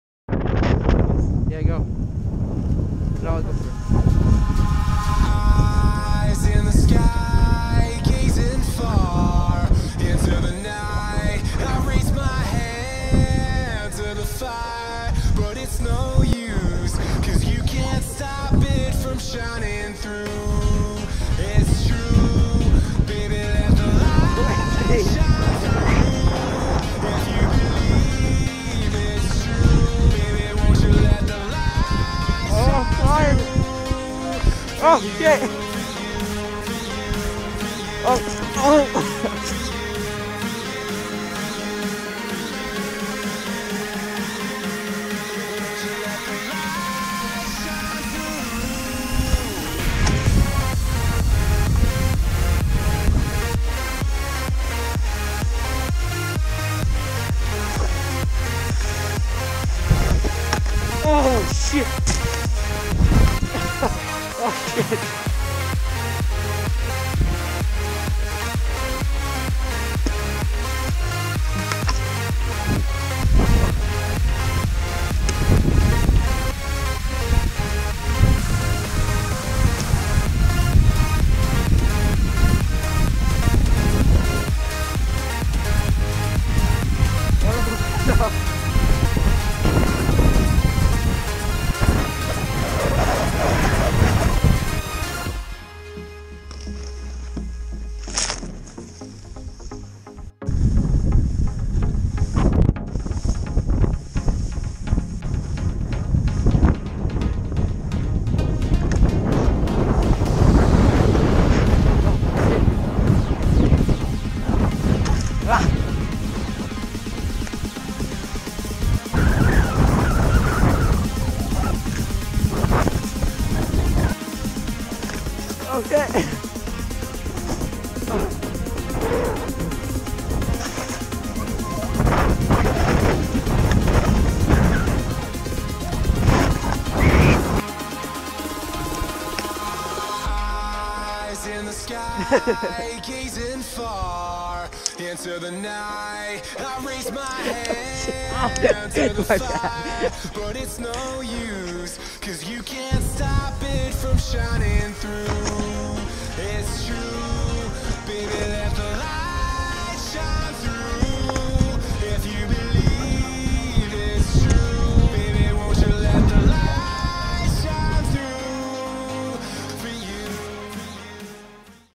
First time back skiing ( At Song Mountain)
first time back of this year, had a great time, more ski edits coming soon.